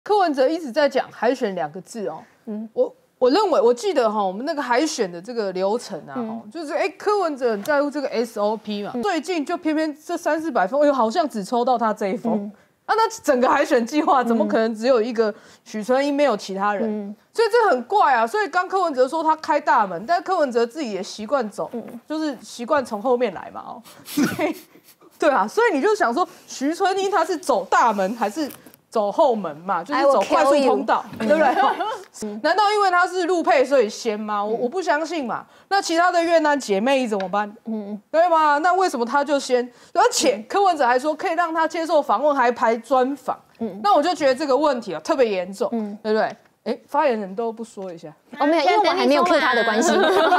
海選開大門 吳靜怡 嗆常走後門
來賓：
立委參選人(民) 吳崢
立法委員(國) 陳玉珍
資深政治記者 尚毅夫
民眾黨發言人 楊寶楨
時事評論員 吳靜怡
資深媒體人 康仁俊
台北市議員(民) 林亮君